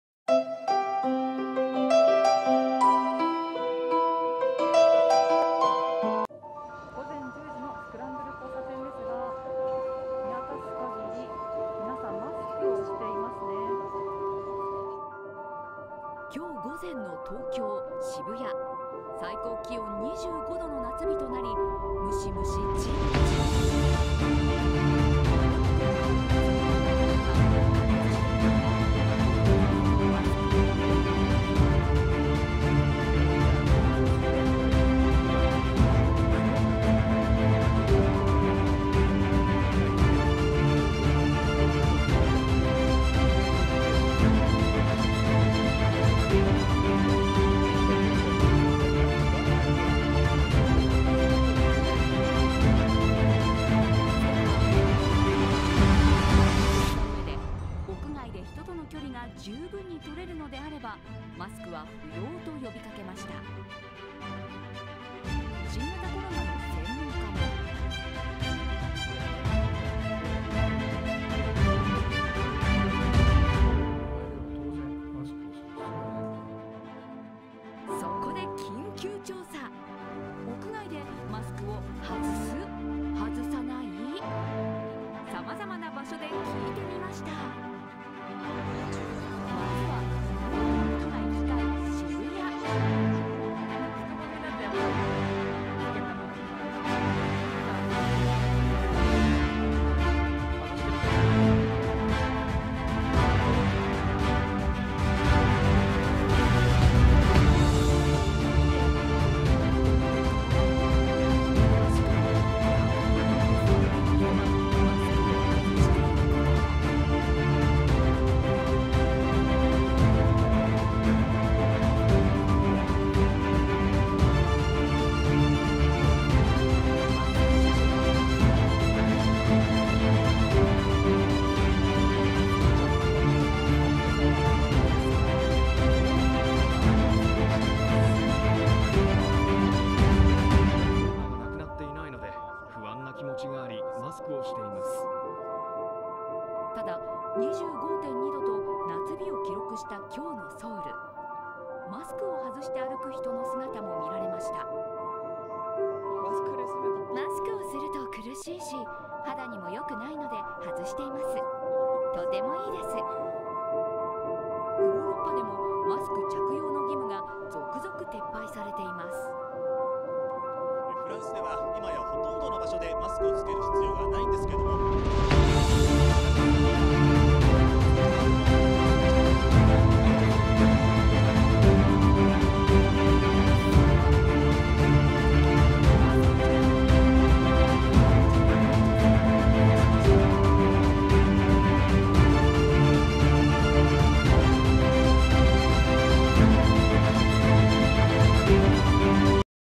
Di Jepang warga sudah boleh melepas masker di luar ruangan???
Menyambut musim panas, suhu di beberapa daerah di Jepang sudah mulai meningkat dan kekhawatiran terhadap dehidrasi meningkat. Melihat hal ini Pemerintahan Perdana Menteri Kishida Fumio mulai menyerukan kepada warga Jepang untuk melepas masker ketika berada diluar ruangan.
Namun untuk bisa melepas masker warga Jepang harus memenuhi syarat yaitu:
1. Menjaga jarak aman 2 meter
2. Mengurangi interkasi dan percakapan
Bersama dengan para ahli Pemerintah menyerukan warga untuk melepas masker demi alasan keamanan karena suhu akan semakin tinggi apabila memasuki bulan 6 karena akan masuk musim panas
Sumber video asli :